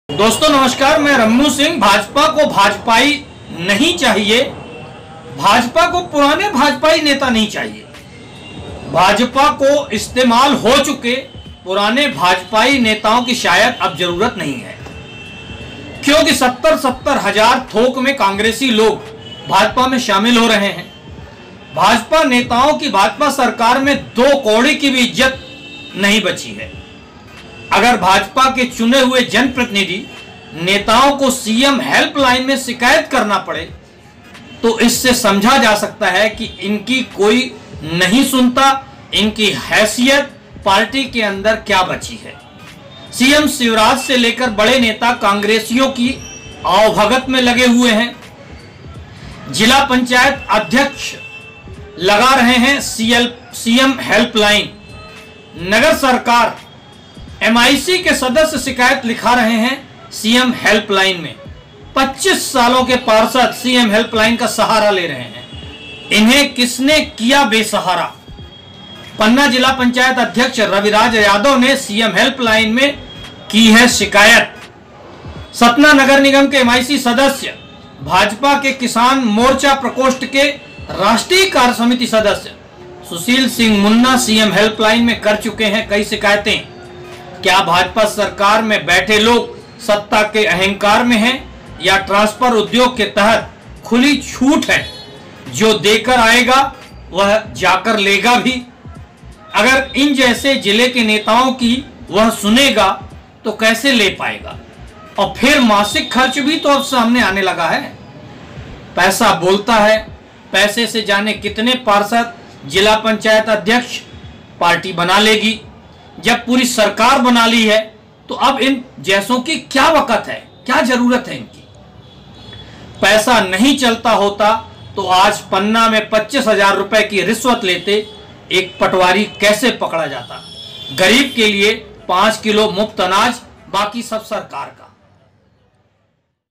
पुराने इस्तेमाल हो चुके भाजपा नेताओं की हैसियत//RammuSingh
भाजपा के जो पुराने दिग्गज नेता हैं उनकी हैसियत यह बची है कि उनकी जिले के अधिकारी कोई बात सुनने को तैयार नहीं है मजबूरी में अब यह बड़े-बड़े पदों पर बैठे हुए भाजपा नेता सीएम हेल्पलाइन का सहारा ले रहे हैं सीएम हेल्पलाइन का सहारा लेने का तात्पर्य यह बता रहा है कि इनकी असली हैसियत असली औकात क्या बची है